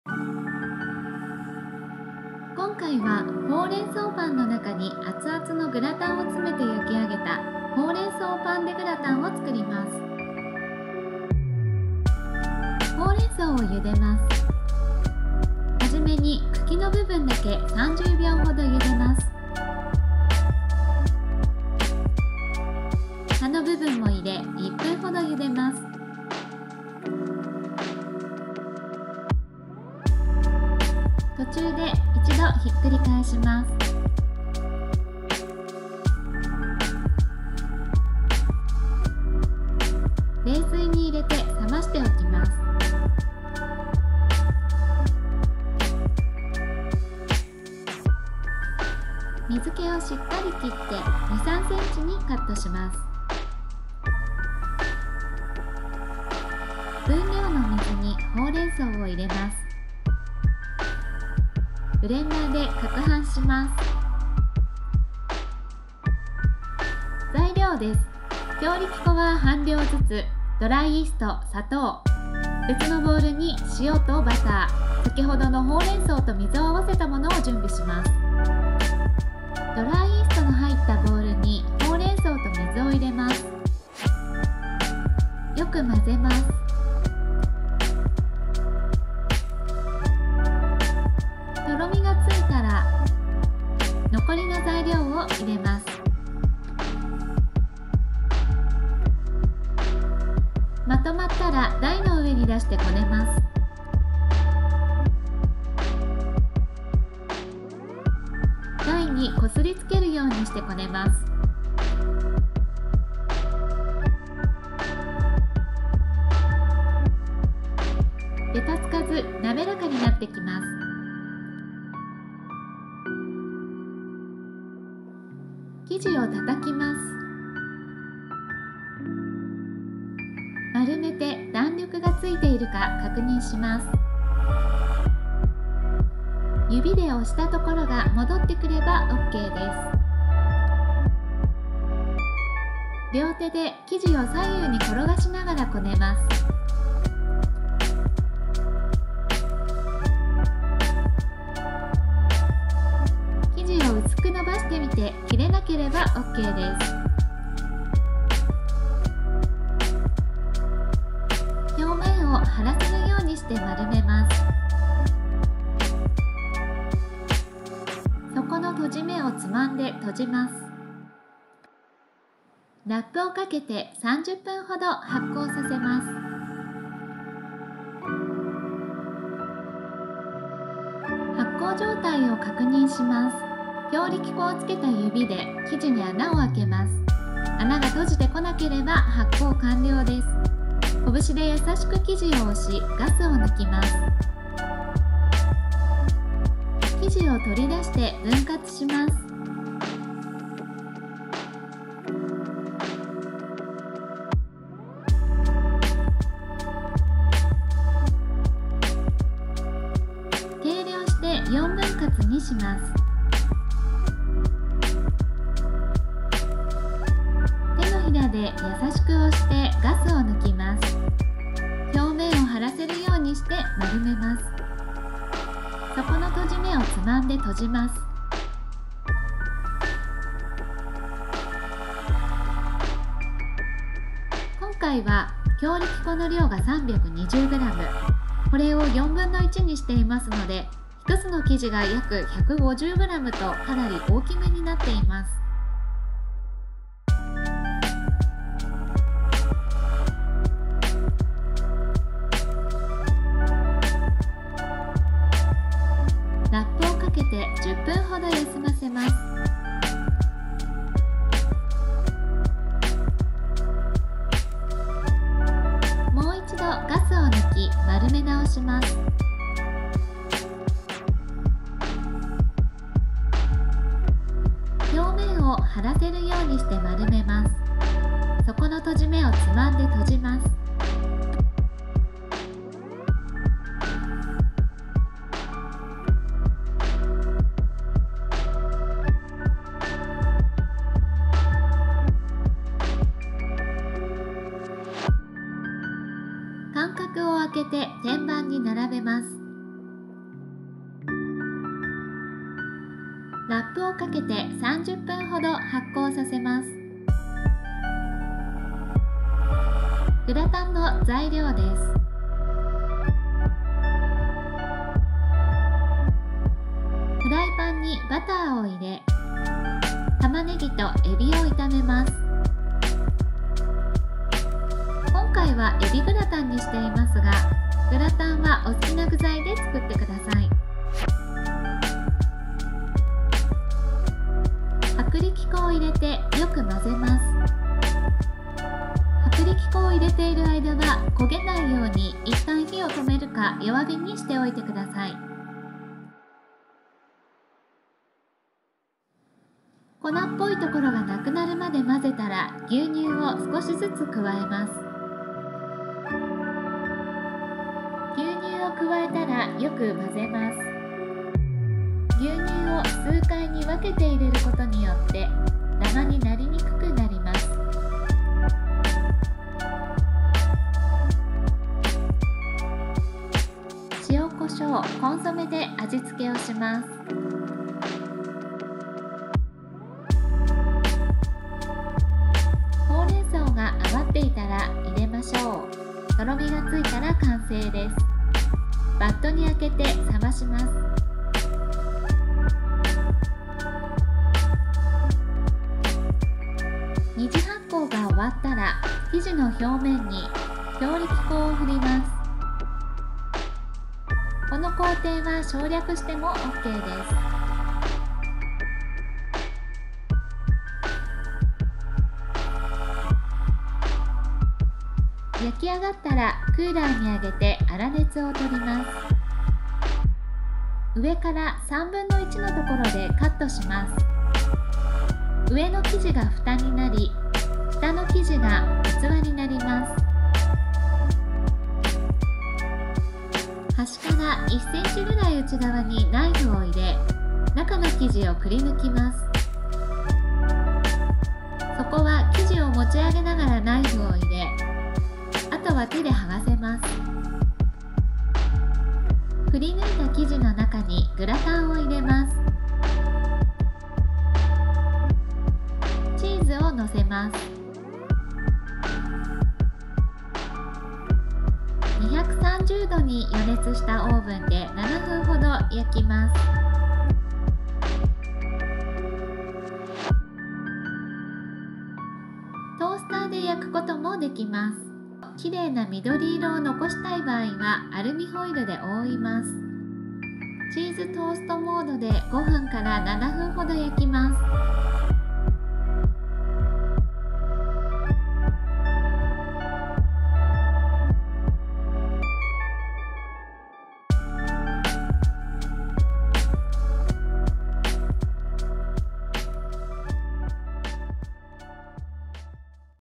『ほうれん草パンdeグラタン』レシピ 冬におすすめ「アツアツ簡単エビグラタン」入り
1:10【ほうれん草パン生地　材料】
強力粉　320g
ドライイースト　5g
砂糖　20g
水　170～190g
塩　4g
無塩バター　20g
ほうれん草　50g

5:10　【エビグラタン　材料】
エビ　150g～160g
玉ねぎ　100g
薄力粉　30g
バター　30g
牛乳　320g
塩コショウ　少々
コンソメ　小さじ1

この他に余ったゆでほうれん草を入れています。

0:12　ほうれん草を茹でる
0:57　ほうれん草をブレンダーで撹拌する
1:17　混ぜる
1:45　こねる
2:43　丸める
2:52　一次発酵
3:01　フィンガーテスト
3:06　ガス抜き
3:22　分割
3:38　丸め
4:15　ベンチタイム
4:22　成形
4:59　二次発酵
5:10　エビグラタンを作る
6:35　焼成
7:00　パンをくり抜く
7:27　グラタンを詰める
7:38　もう一度焼く

作り方の詳細、材料、解説などはブログをご覧ください